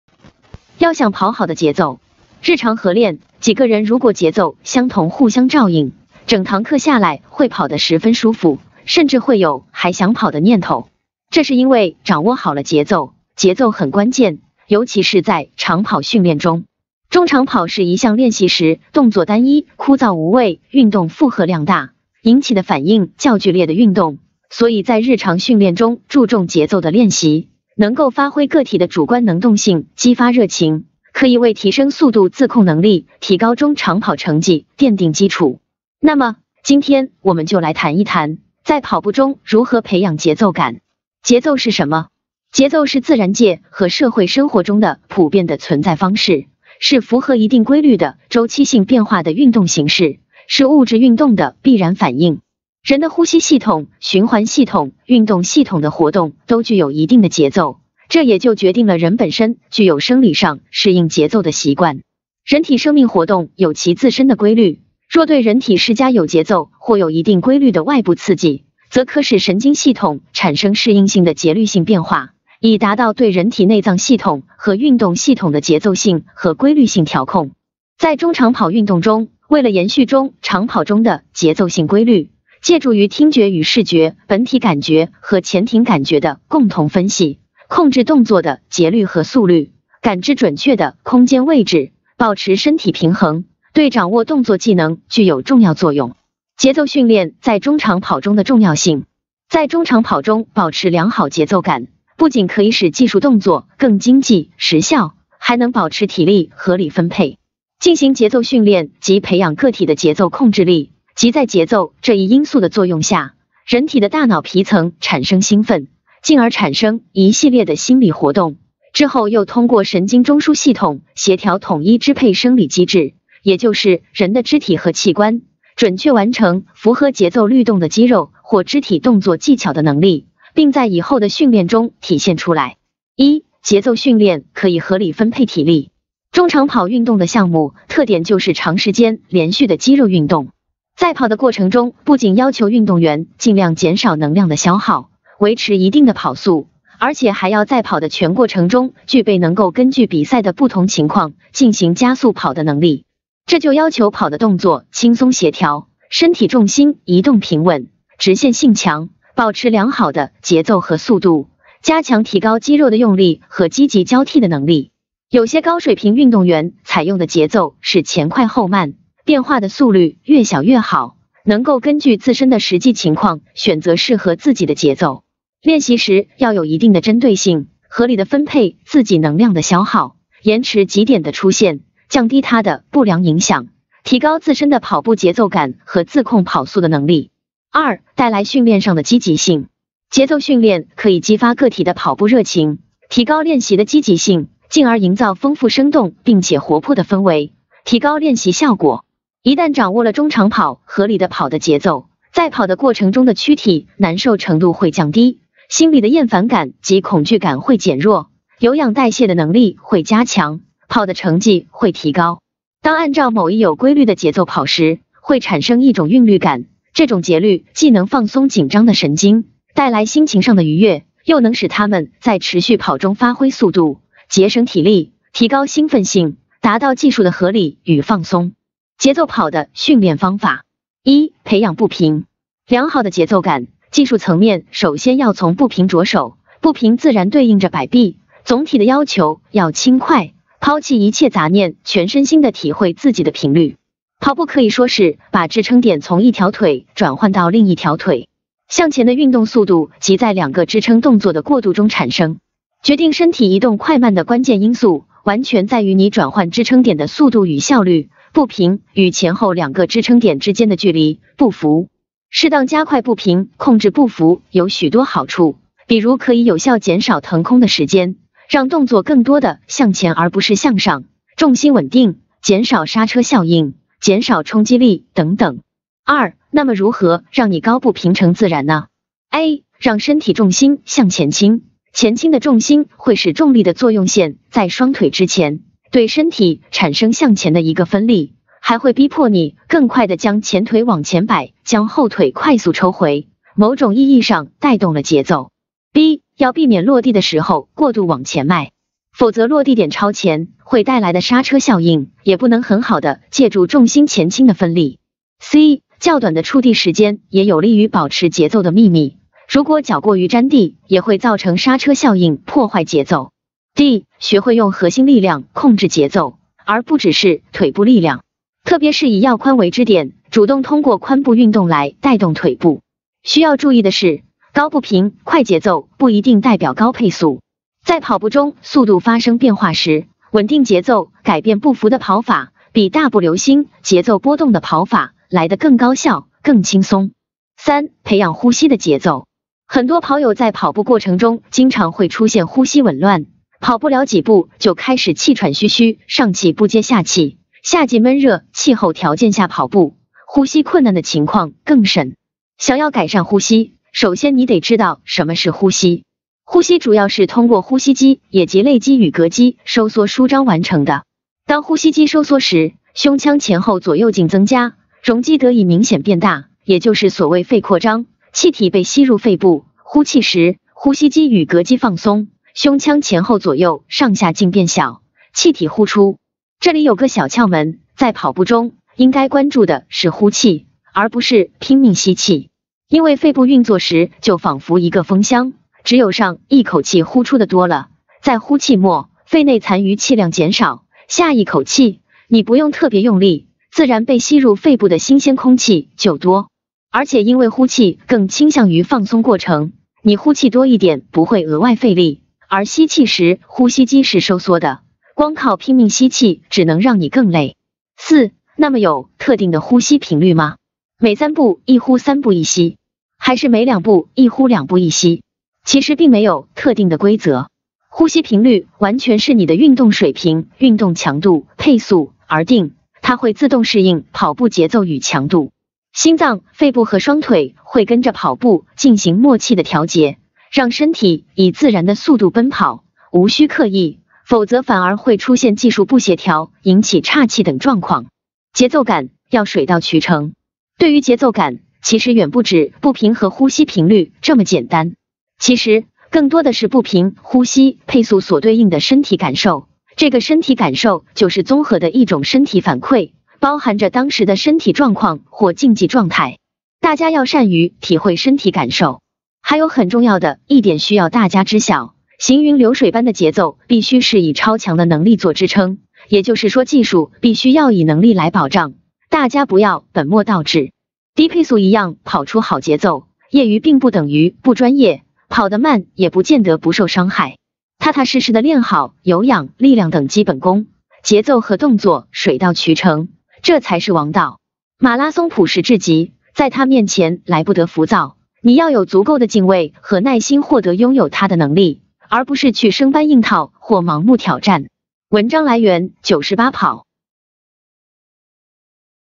（國語版）要想跑好的節奏
日常合練，幾個人如果節奏相同互相照應，整堂課下來會跑的十分舒服，甚至會有還想跑的念頭。這是因為掌握好了節奏，節奏很關鍵，尤其是在長跑訓練中。

If several people keep the same rhythm and take care of each other, they will run very comfortably after the whole class, and even want to run. This is because rhythm is critical, especially in long-distance training.